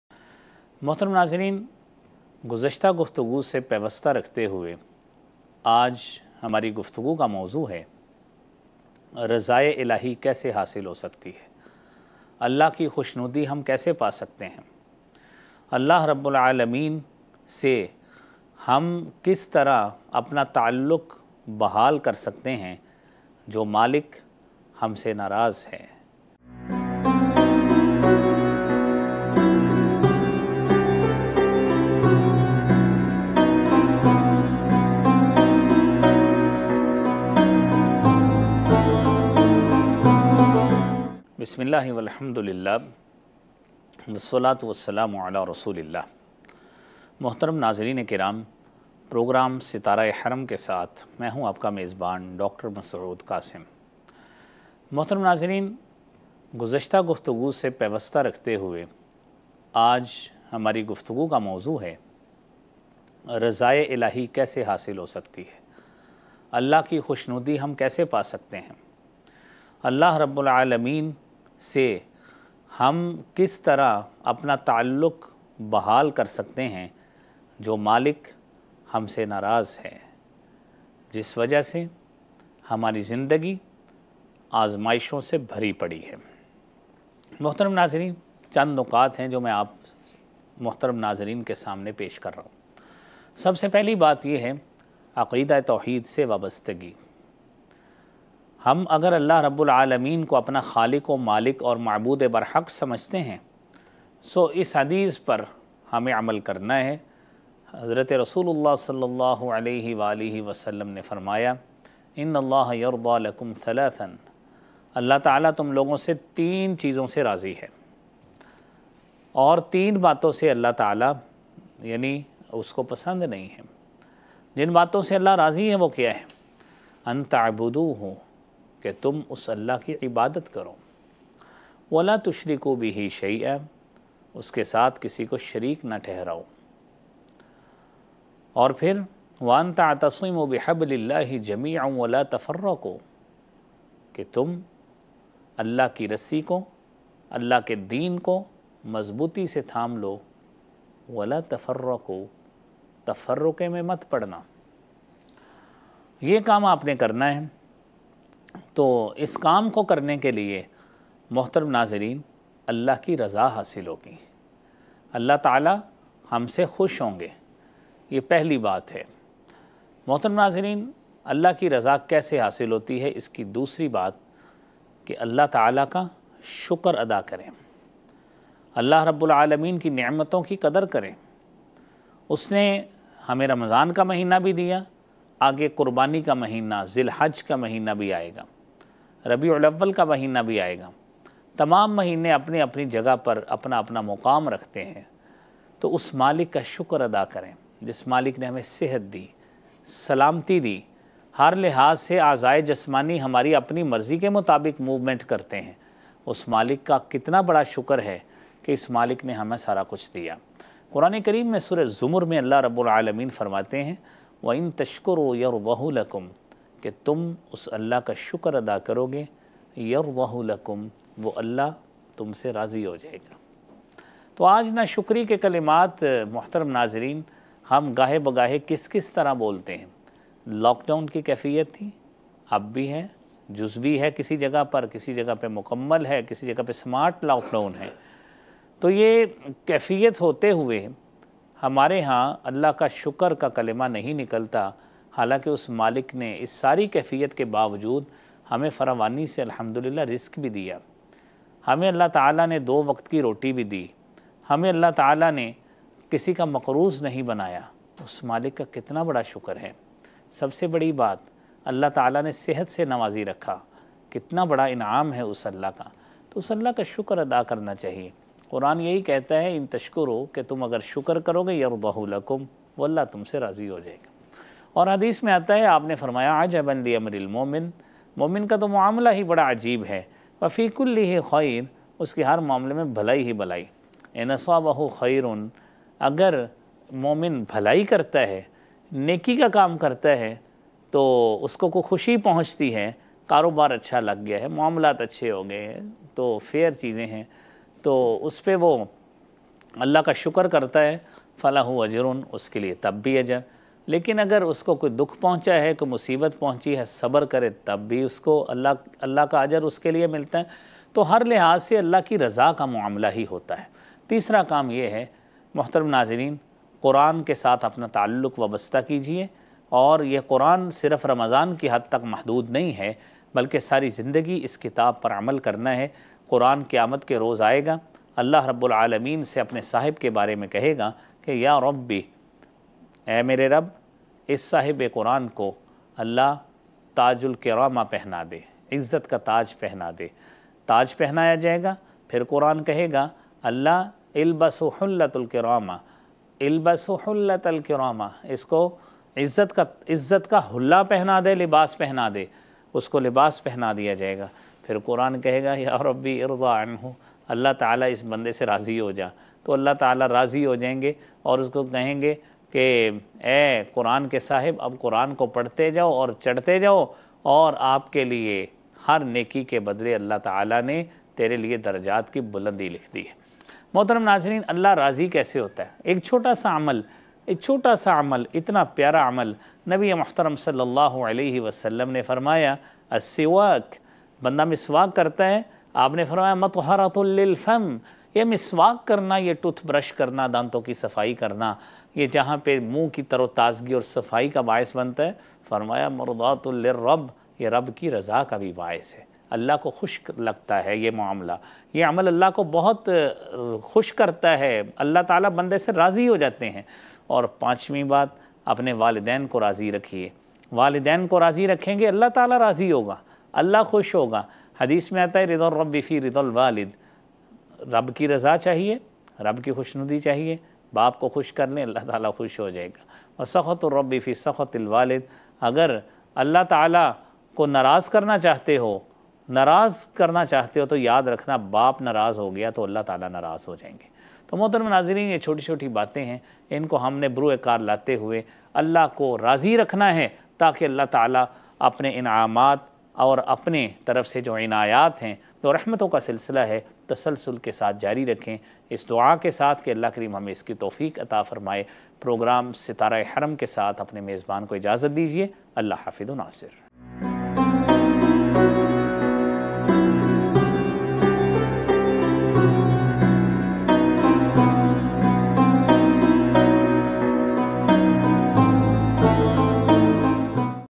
Allah Ki Raza ka hasool kaisay Mumkin? || Sitara E haram
Allah rab ul izat ki khushnudi ham kaisay hasil kr saktay hain? Jo Rab hm se naraz he.  Jiski waja se hamari Zindagi azmaishoon se bhari parhi he.